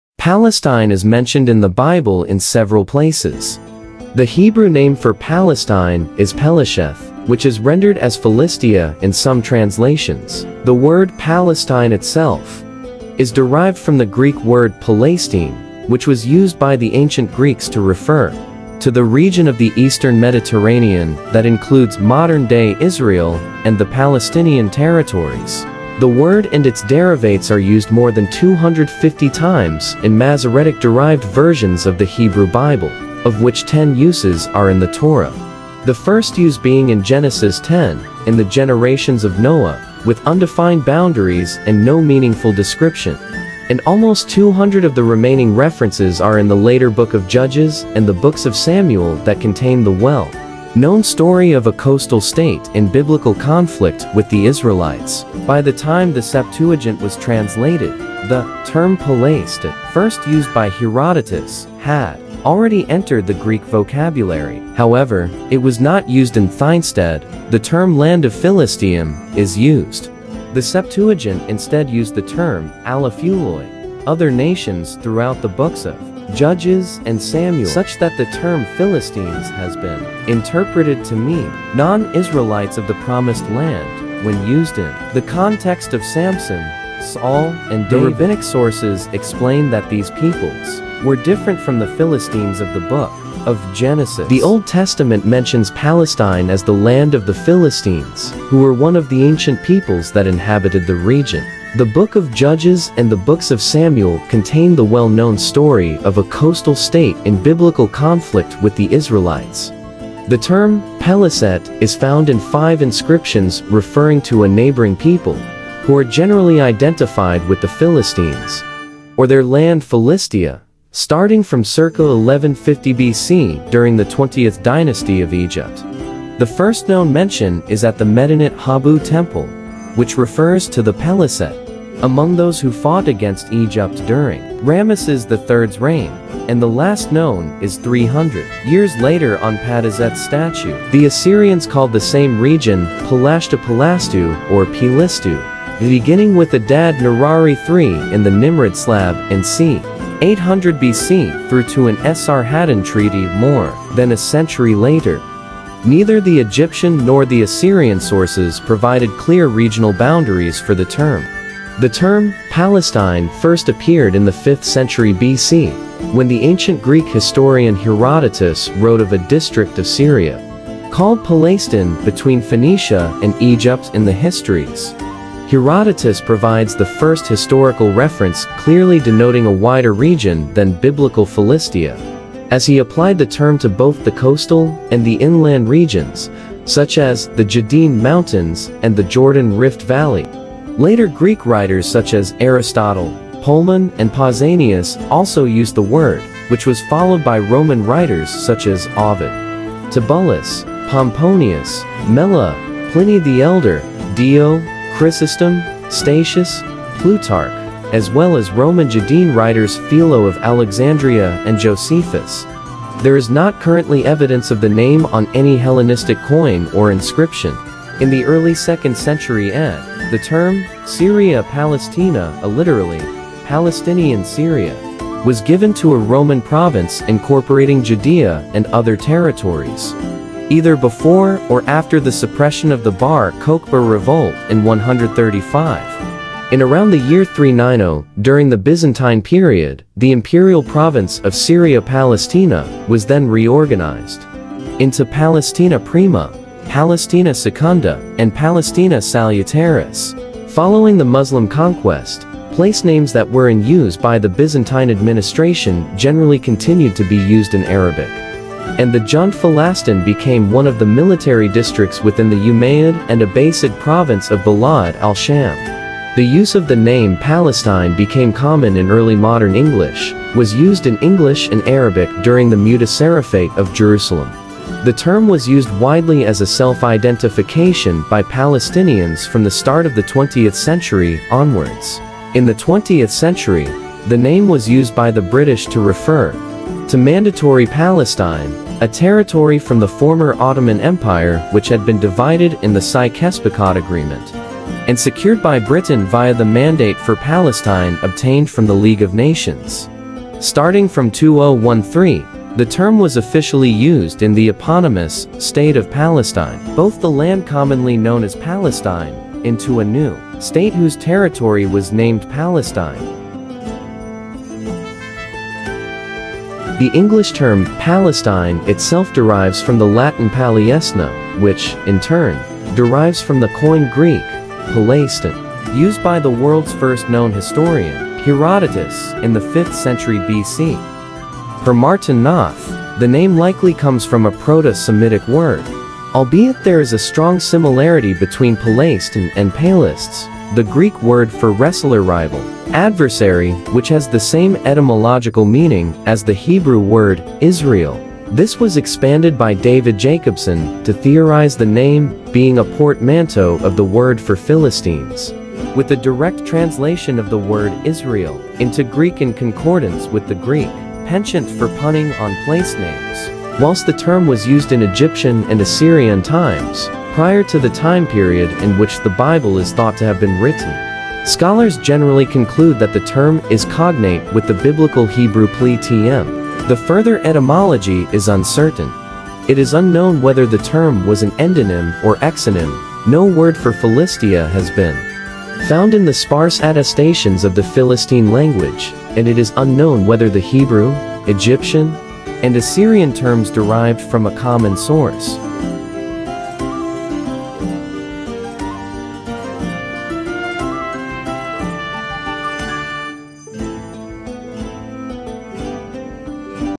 The Origins of Palestine: A Biblical and Historical Account
Palestine is mentioned in the Bible in several places. The Hebrew name for Palestine is Pelesheth, which is rendered as “Philistia” in some translations 1. The word “Palestine” itself is derived from the Greek word “Palaistine,” which was used by the ancient Greeks to refer to the region of the eastern Mediterranean that includes modern-day Israel and the Palestinian territories 2.
The Old Testament mentions Palestine as the land of the Philistines, who were one of the ancient peoples that inhabited the region 13. The Book of Judges and the Books of Samuel contain the well-known story of a coastal state in biblical conflict with the Israelites

The term "Peleset" (transliterated from hieroglyphs as P-r-s-t) is found in five inscriptions referring to a neighboring people, who are generally identified with the Philistines,[2] or their land Philistia, starting from circa 1150 BCE during the Twentieth Dynasty of Egypt. The first known mention is at the Medinet Habu temple which refers to the Peleset among those who fought against Egypt during Ramesses III's reign,[3] and the last known is 300 years later on Padiiset's Statue. The Assyrians called the same region "Palashtu/Palastu" or "Pilistu," beginning with Adad-nirari III in the Nimrud Slab in c. 800 BCE through to an Esarhaddon treaty more than a century later.[4][5] Neither the Egyptian nor the Assyrian sources provided clear regional boundaries for the term.[6]
The term "Palestine" first appeared in the 5th century BCE when the ancient Greek historian Herodotus wrote of a "district of Syria, called Palaistinê" between Phoenicia and Egypt in The Histories.[7] Herodotus provides the first historical reference clearly denoting a wider region than biblical Philistia, as he applied the term to both the coastal and the inland regions such as the Judean Mountains and the Jordan Rift Valley.[8][9][10][11] Later Greek writers such as Aristotle, Polemon and Pausanias also used the word, which was followed by Roman writers such as Ovid, Tibullus, Pomponius Mela, Pliny the Elder, Dio Chrysostom, Statius, Plutarch as well as Roman Judean writers Philo of Alexandria and Josephus.[12] There is not currently evidence of the name on any Hellenistic coin or inscription.[13]
In the early 2nd century CE, the term "Syria Palaestina"[a] (literally, "Palestinian Syria"[14][15]) was given to a Roman province incorporating Judaea and other territories, either before or after the suppression of the Bar Kokhba revolt in 135.[16][17][18][19] In around the year 390, during the Byzantine period, the imperial province of Syria Palaestina was then reorganized into Palaestina Prima, Palaestina Secunda[20] and Palaestina Salutaris.[20] Following the Muslim conquest, place names that were in use by the Byzantine administration generally continued to be used in Arabic,[4][21] and the Jund Filastin became one of the military districts within the Umayyad and Abbasid province of Bilad al-Sham.[22]
The use of the name "Palestine" became common in Early Modern English,[23] was used in English and Arabic during the Mutasarrifate of Jerusalem. The term was used widely as a self-identification by Palestinians from the start of the 20th century onwards.[24] In the 20th century the name was used by the British to refer to "Mandatory Palestine," a territory from the former Ottoman Empire which had been divided in the Sykes–Picot Agreement and secured by Britain via the Mandate for Palestine obtained from the League of Nations.[25] Starting from 2013, the term was officially used in the eponymous "State of Palestine."[26] Both incorporated geographic regions from the land commonly known as Palestine, into a new state whose territory was named Palestine.
The English term "Palestine" itself derives from the Latin Palaestīna,[27] which, in turn, derives from the Koine Greek Παλαιστῑ́νη, Palaistī́nē, used by the world's first known historian, Herodotus, in the 5th century BCE.[7] Per Martin Noth, the name likely comes from a proto-Semitic word, albeit there is a strong similarity between Palaistī́nē and palaistês, the Greek word for "wrestler/rival/adversary", which has the same etymological meaning as the Hebrew word "Israel."[28] This was expanded by David Jacobson to theorize the name being a portmanteau of the word for Philistines with a direct translation of the word Israel into Greek (in concordance with the Greek penchant for punning on place names.)[29][